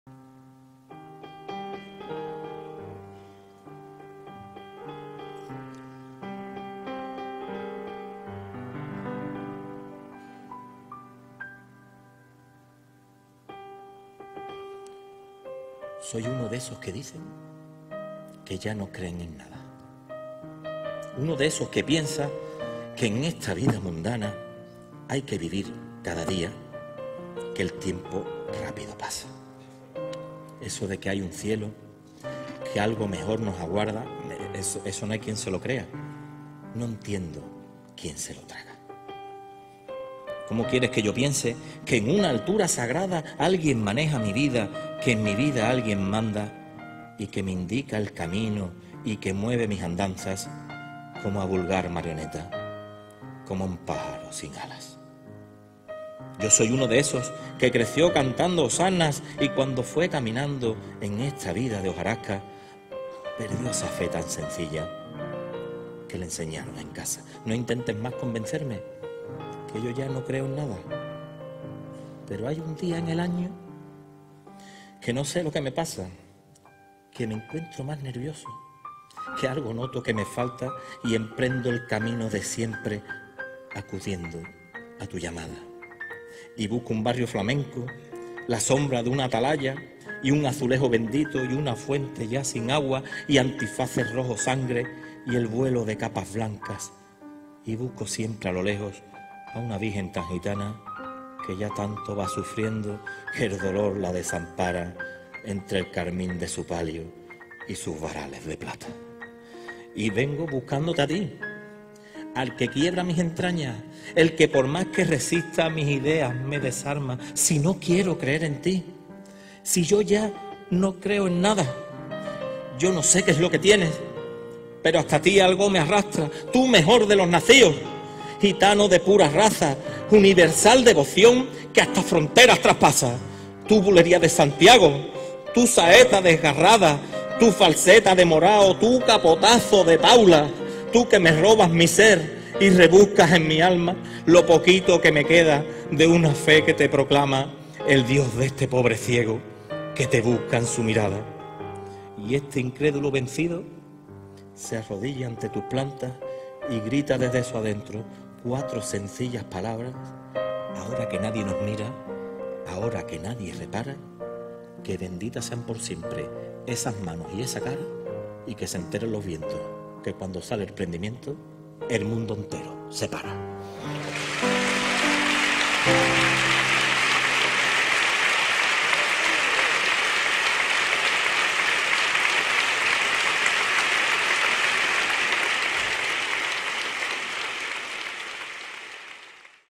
Benditas sean por siempre esas manos y esa cara Prendimiento
Tú que me robas mi ser y rebuscas en mi alma, lo poquito que me queda de una fe que te proclama el Dios de este pobre ciego que te busca en su mirada. Prendimiento
Versos de Ángel Luis Rodríguez Aguilocho; Pregonero de la Semana Santa Jerez 2019.
Ntro. Padre Jesús del Prendimiento Ntra. Sra. del Desamparo.